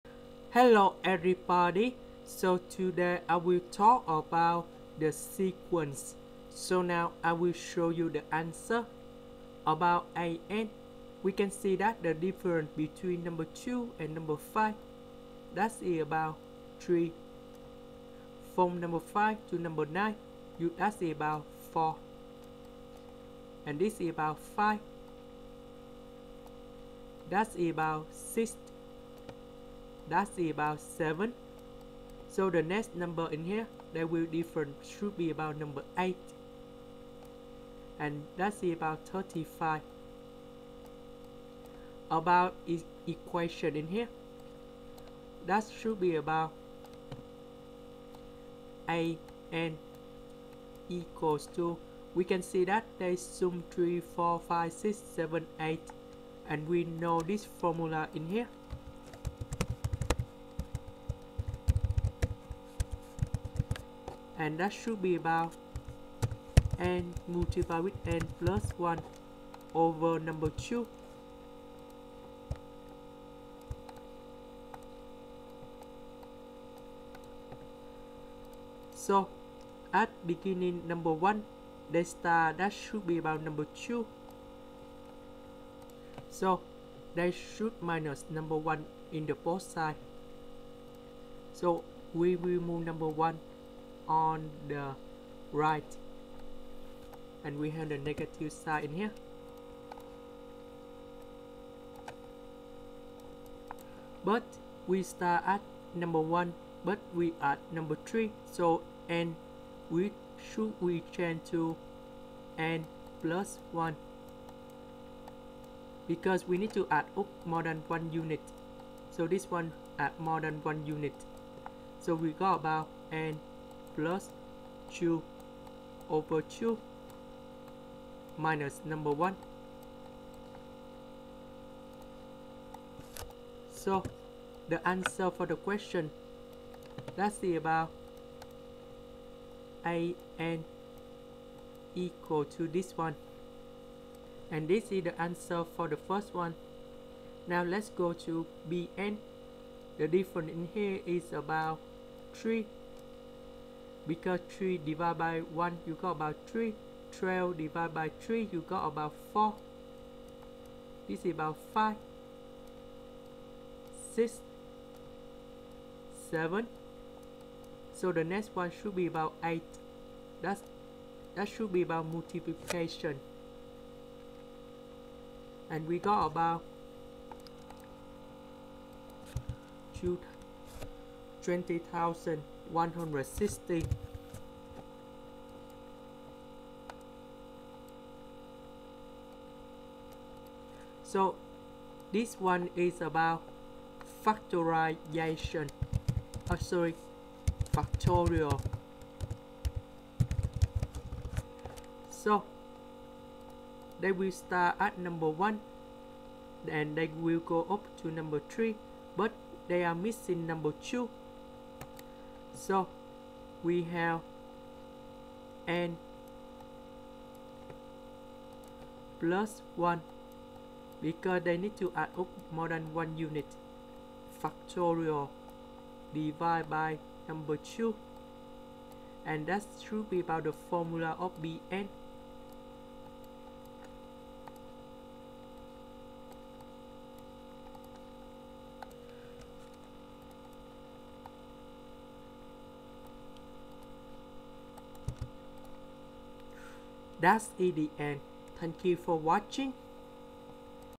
Algebra Help: Sequence and Series: Problem A - International Youth Math Challenge
Here is the technique to find the sequence and series below